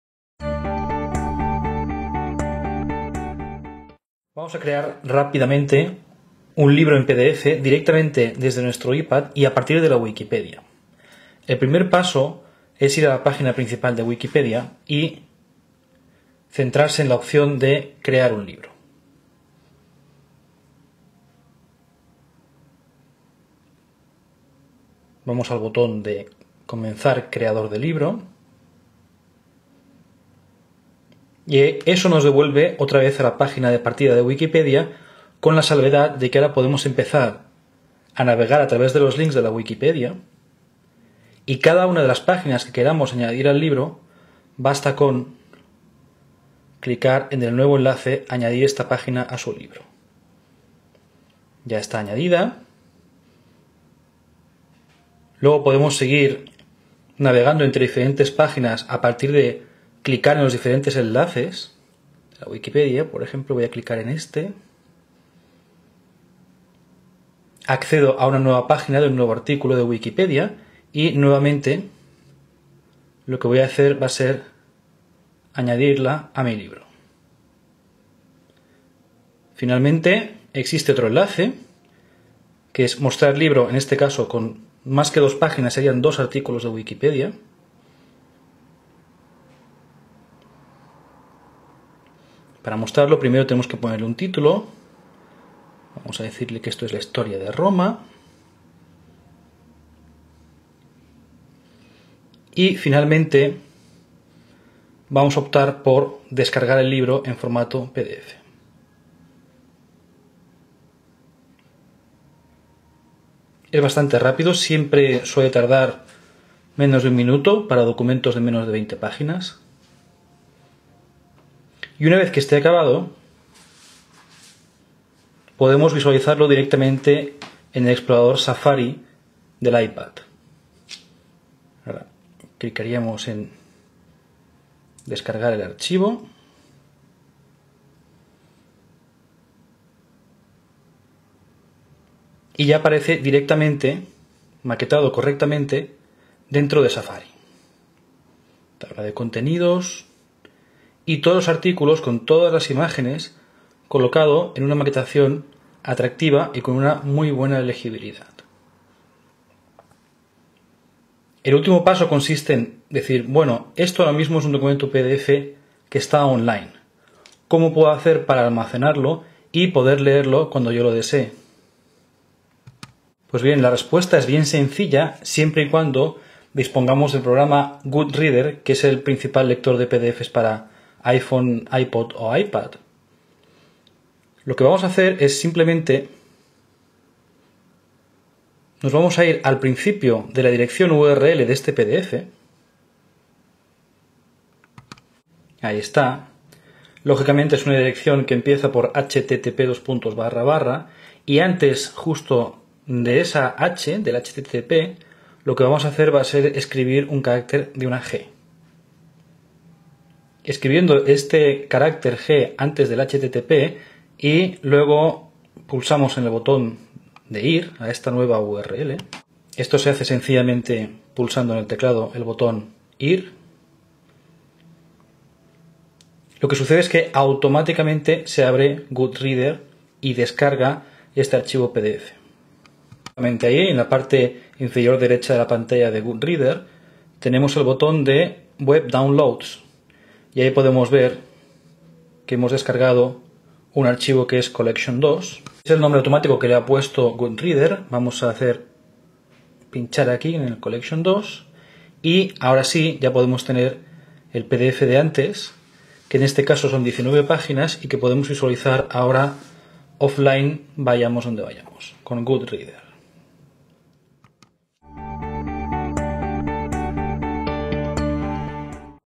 Hacerse un PDF desde la Wikipedia solo con el iPad
Cómo navegar desde tu iPad por la Wikipedia e ir "pescando" artículos capturándolos en PDF para poder leerlos posteriormente offline utilizando GoodReader para iPad. (funciona igual para iPhone o iPod touch)